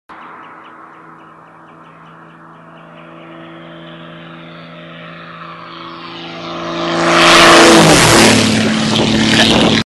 High speed lowpass Extra 300L at Teuge Airport (Ewald Polinder Aerobatics)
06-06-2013, High speed lowpass inside the camera!